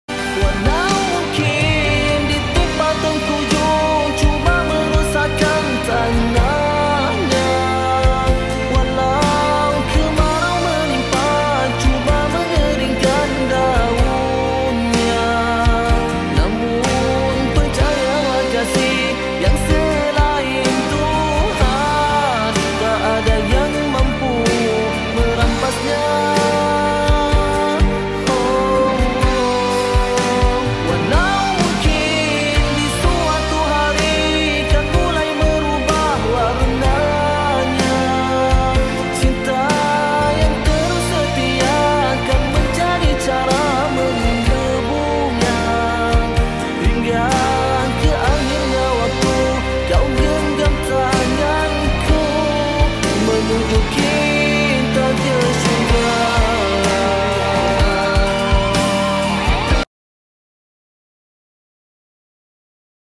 MH17 , 17.7.2014-2024
Tragedi yang menyentap seluruh rakyat Malaysia, MH17 turut mengorbankan adik ipar cikgu Anne , Mohd Ali b Md Salim yg pulang utk bercuti menyambut aidilfitri.
kredit to my sil atas video ini. Alfatihah utk semua mangsa MH17 yg muslim.